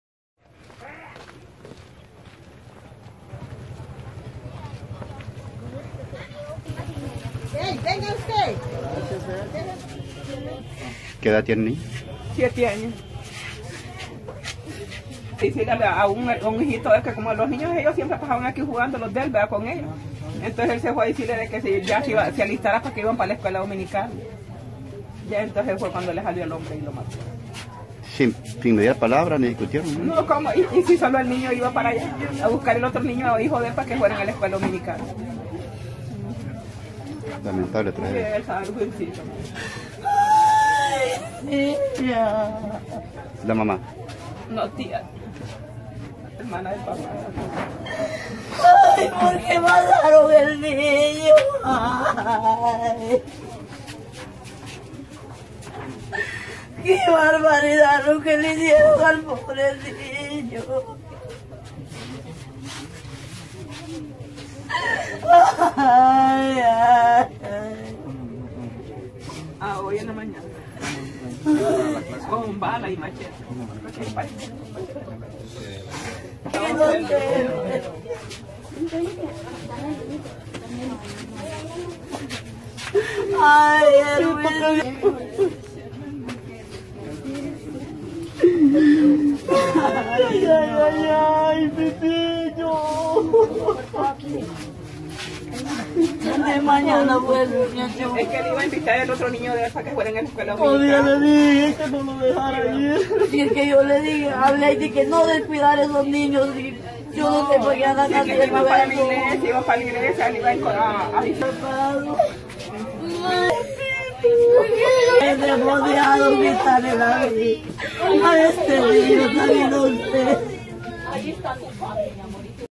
Hombre rechazado por mujer mata a su hijo en Tocoa
Un hombre mató a un niño porque la madre no aceptó sus pretensiones amorosas, en la ciudad de Tocoa, departamento de Colón, zona norte de Honduras.